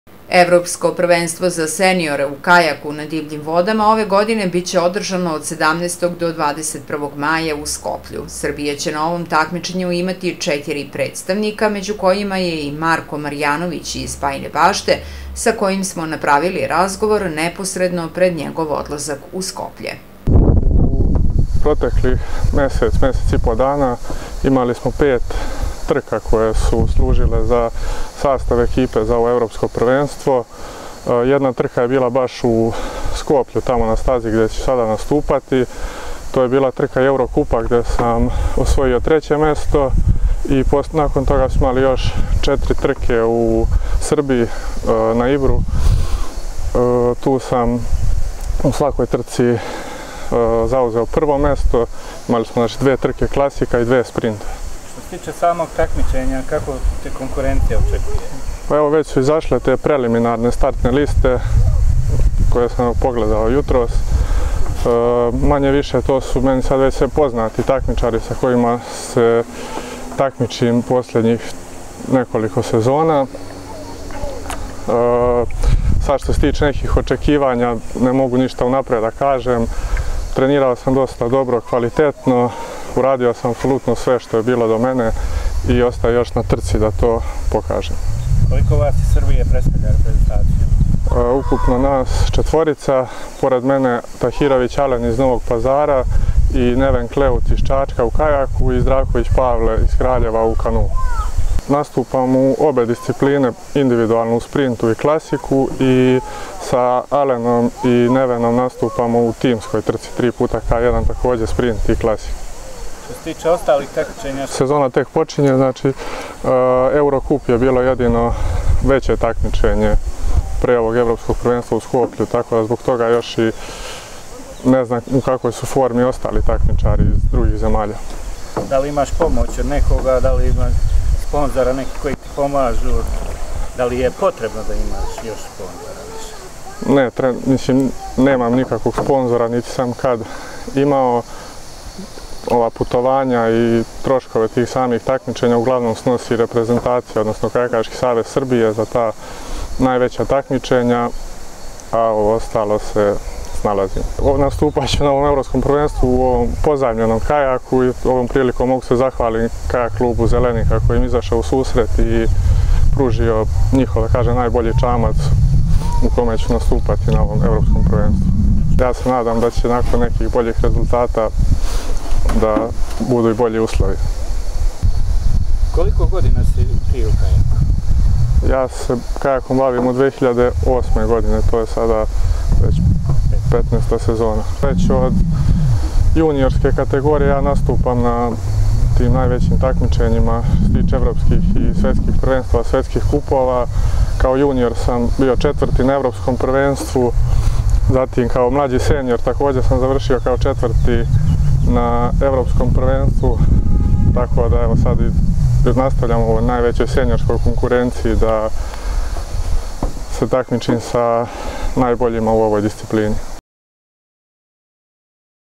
Marko Marjanović na Evropskom prenstvu
Od 17 do 21. maja u Severnoj Makedoniji u Skoplju održava se Evropsko prvenstvo u kajaku na divljim vodama za seniore na kome učestvuje i Bajinobaštanin Marko Marjanović